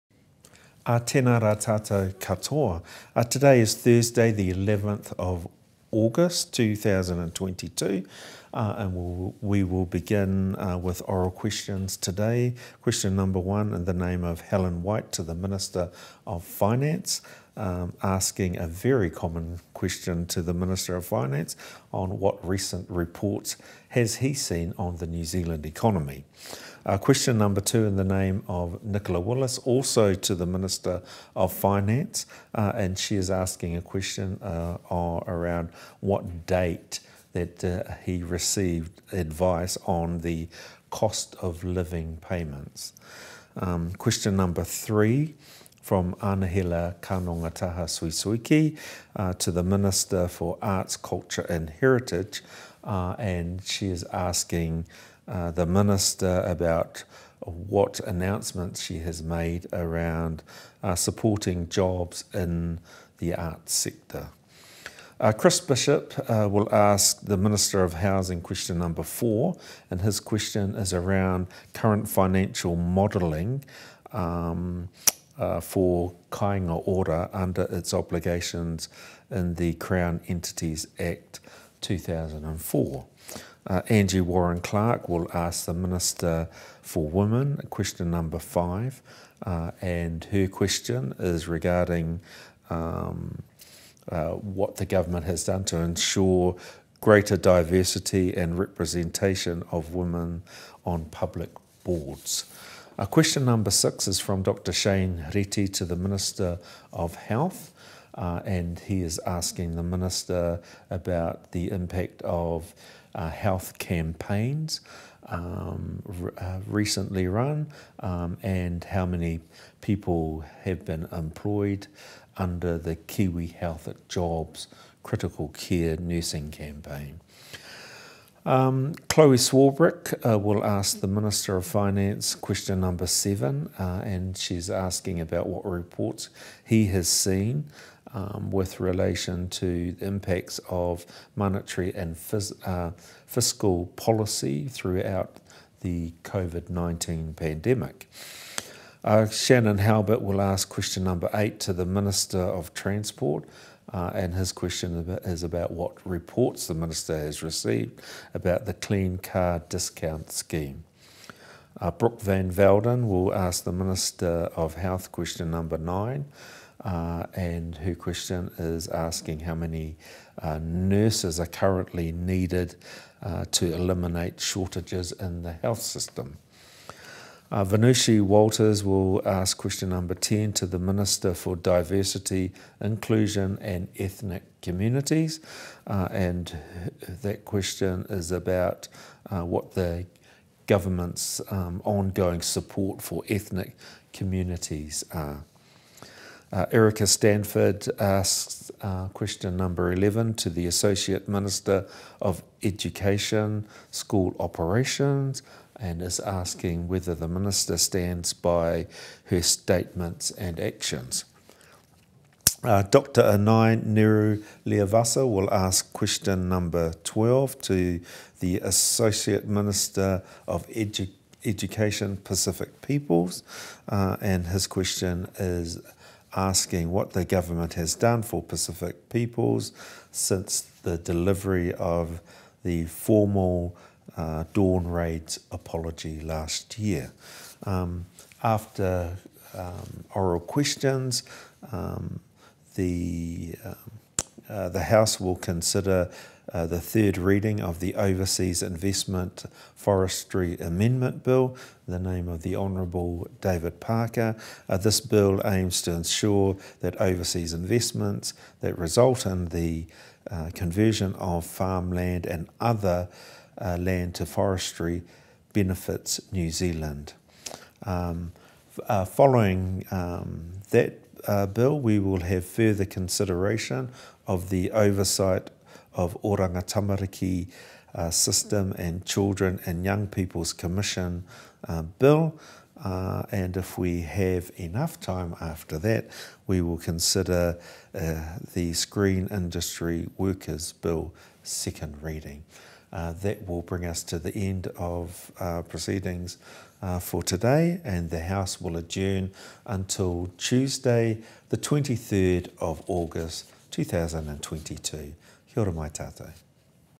Today in the House: for 11 August 2022
The Deputy Speaker of the House, Adrian Rurawhe, talks about what's happening in the House today.